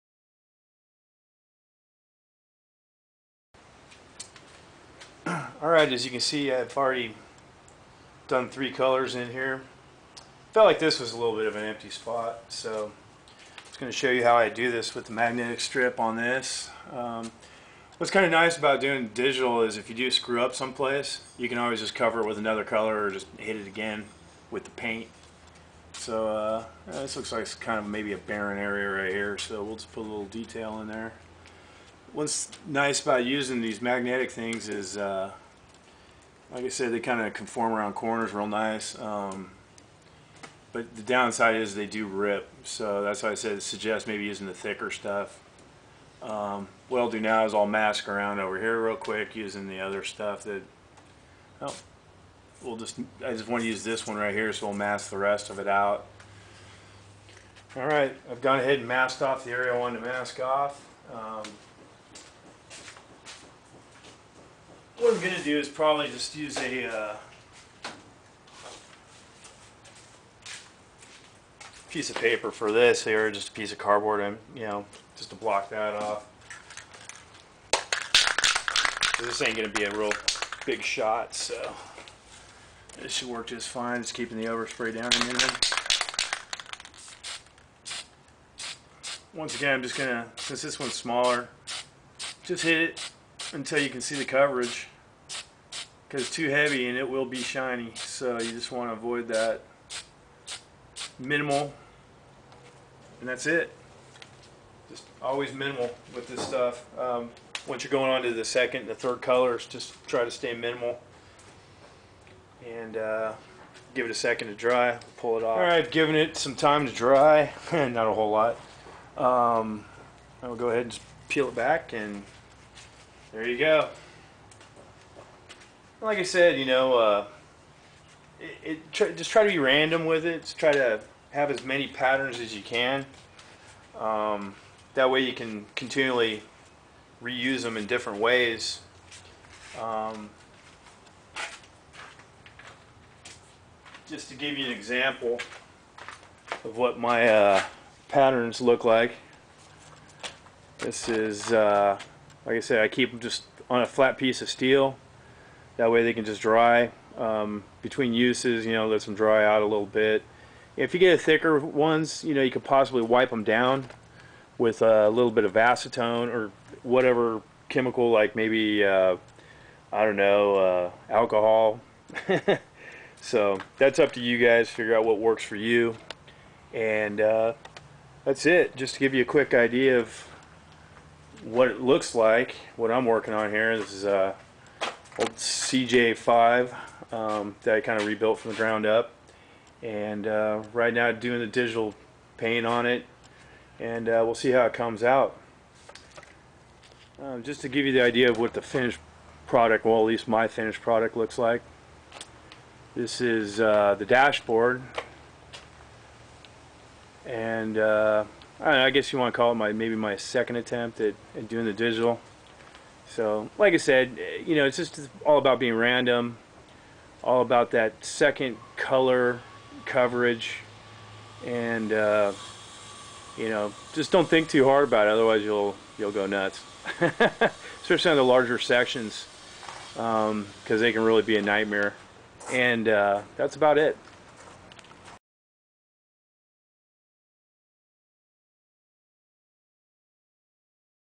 how to paint digital camo 4
how to paint the darker colors

You can see the completed project  in the Dec. 2017 issue of JP Magazine